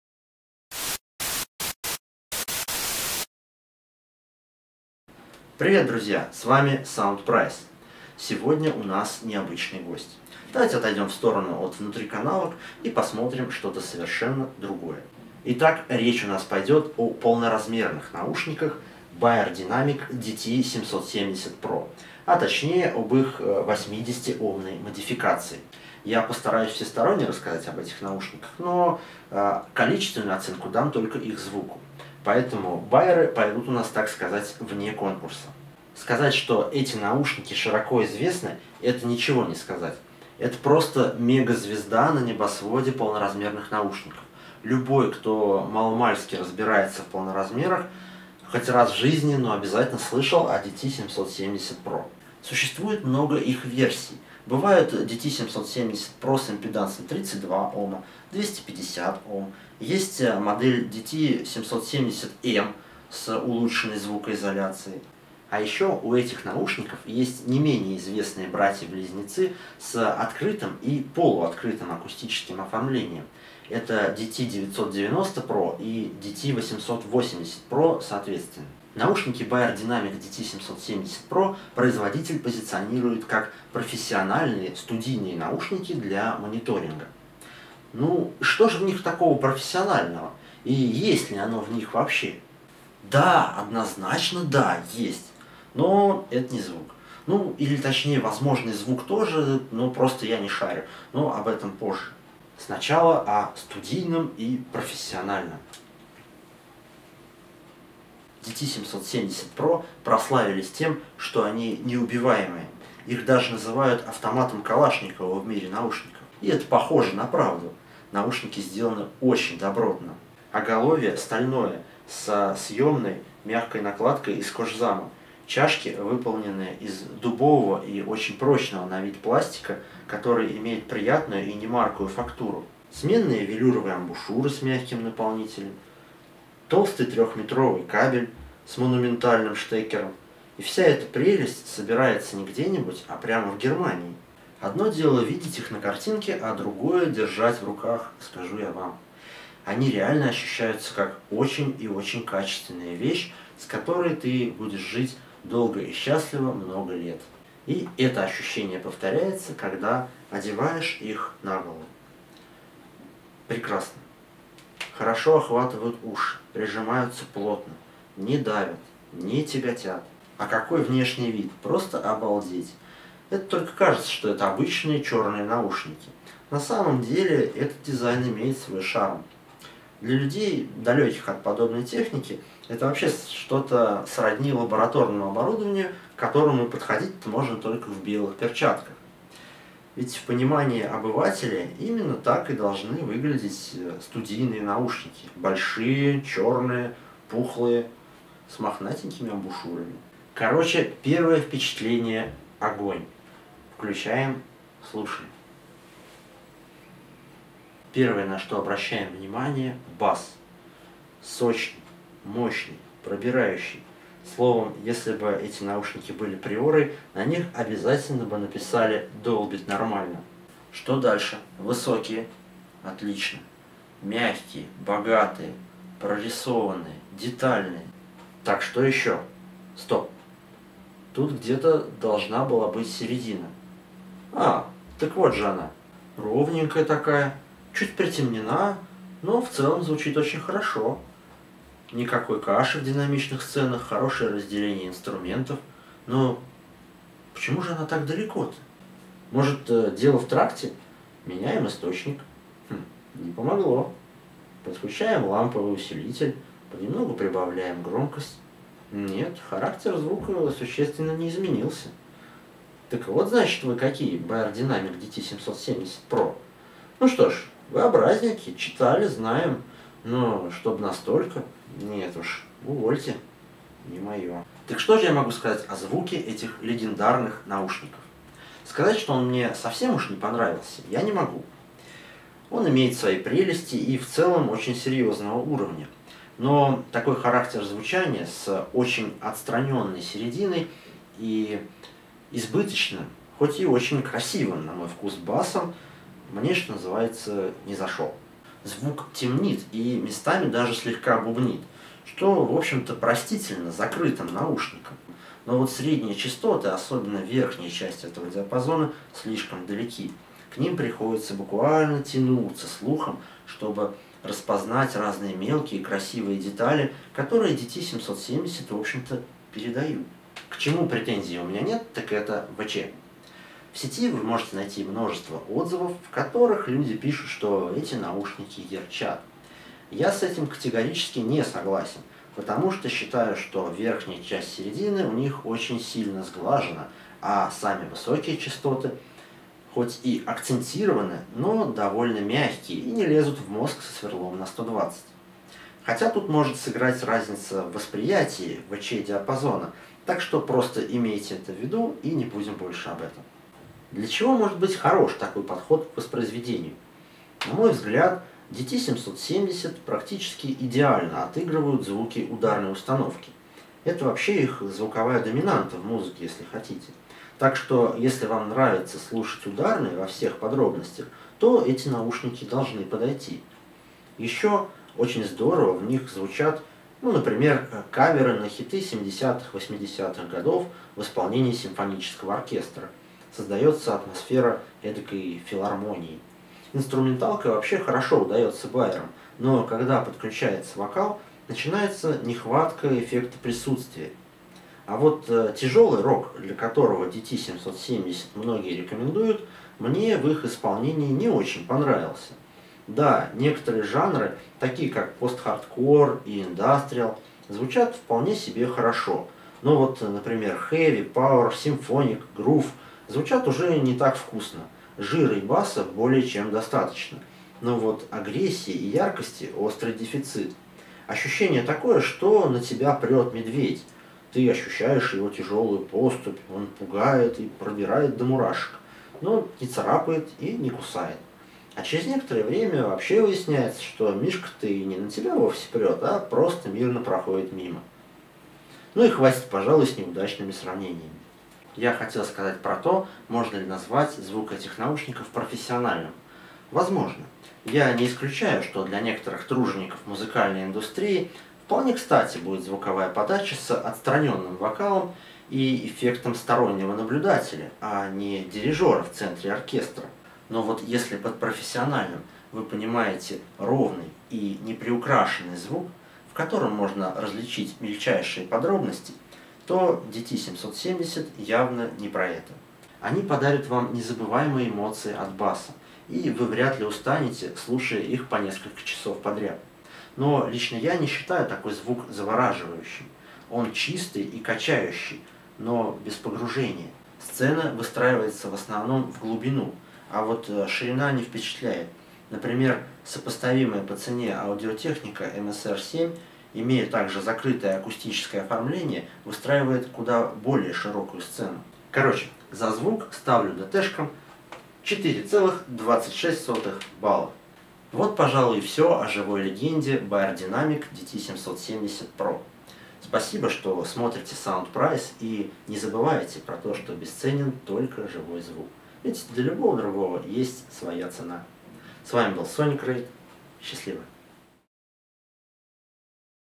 Beyerdynamic DT 770 Pro обзор наушников
Beyerdynamic DT 770 Pro: внешний вид, комплектация, качество, звук